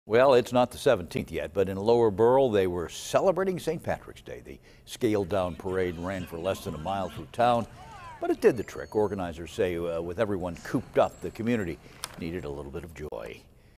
Lower Burrell Group Holds St. Patrick's Day Parade
In Lower Burrell, residents got out to celebrate St. Patrick's Day early with a parade.